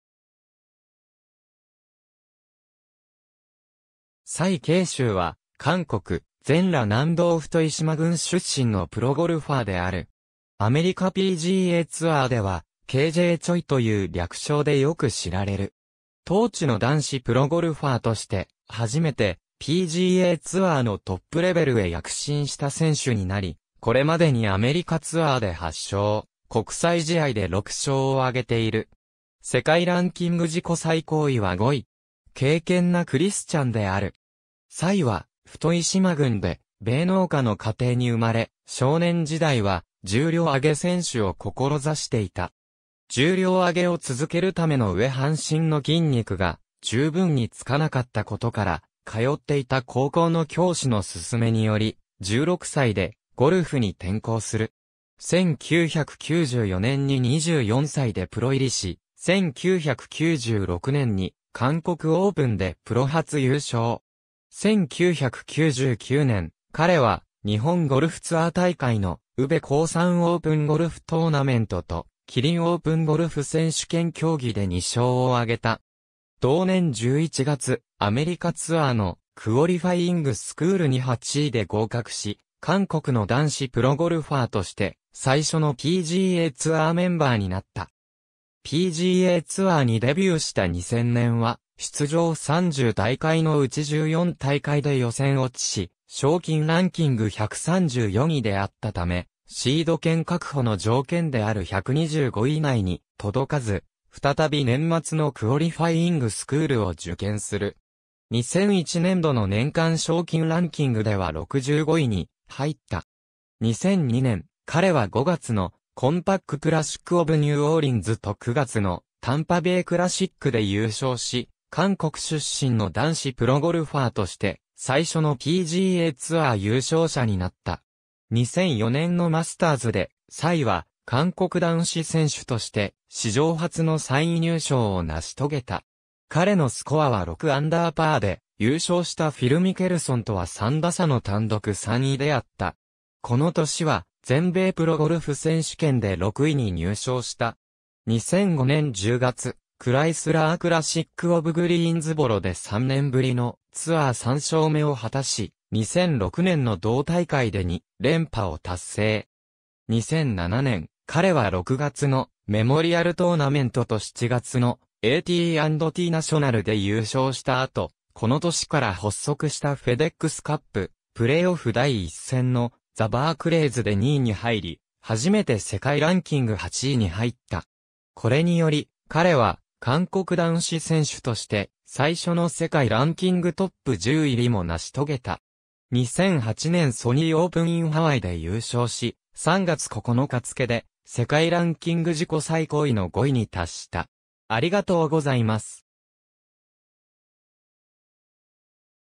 崔京周
崔 京周（チェ・キョンジュ、최경주, Choi Kyung-ju, 1970年5月19日 - ）は、韓国・全羅南道莞島郡出身のプロゴルファーである。
アメリカPGAツアーでは“K. J. Choi”という略称でよく知られる。
当地の男子プロゴルファーとして、初めてPGAツアーのトップレベルへ躍進した選手になり、これまでにアメリカツアーで8勝、国際試合で6勝を挙げている。
世界ランキング自己最高位は5位（2008年3月）。
敬虔なクリスチャンである。
崔は莞島郡で米農家の家庭に生まれ、少年時代は重量挙げ選手を志していた。
重量挙げを続けるための上半身の筋肉が十分につかなかったことから、通っていた高校の教師の勧めにより、16歳でゴルフに転向する。
1994年に24歳でプロ入りし、1996年に「韓国オープン」でプロ初優勝。
1999年、彼は日本ゴルフツアー大会の「宇部興産オープンゴルフトーナメント」（2001年に廃止）と「キリンオープンゴルフ選手権競技」で2勝を挙げた。
同年11月、アメリカツアーの「クオリファイイング・スクール」（シード権取得のための資格試験）に8位で合格し、韓国の男子プロゴルファーとして最初のPGAツアーメンバーになった。
PGAツアーにデビューした2000年は出場30大会のうち14大会で予選落ちし、賞金ランキング134位であったため、シード権確保の条件である125位以内に届かず、再び年末のクオリファイイング・スクールを受験する。
2001年度の年間賞金ランキングでは65位に入った。
2002年、彼は5月の「コンパック・クラシック・オブ・ニューオーリンズ」と9月の「タンパベイ・クラシック」で優勝し、韓国出身の男子プロゴルファーとして最初のPGAツアー優勝者になった。
2004年のマスターズで、崔は韓国男子選手として史上初の3位入賞を成し遂げた。
彼のスコアは 6 アンダーパー（-6, 282ストローク）で、優勝したフィル・ミケルソンとは3打差の単独3位であった。
この年は全米プロゴルフ選手権で6位に入賞した。
2005年10月、「クライスラー・クラシック・オブ・グリーンズボロ」で3年ぶりのツアー3勝目を果たし、2006年の同大会で2連覇を達成。
2007年、彼は6月の「メモリアル・トーナメント」と7月の「AT&Tナショナル」で優勝した後、この年から発足したフェデックスカップ・プレーオフ第1戦の「ザ・バークレイズ」で2位に入り、初めて世界ランキング8位に入った。
これにより、彼は韓国男子選手として最初の世界ランキングトップ10入りも成し遂げた。
2008年ソニーオープン・イン・ハワイで優勝し、3月9日付で世界ランキング自己最高位の5位に達した。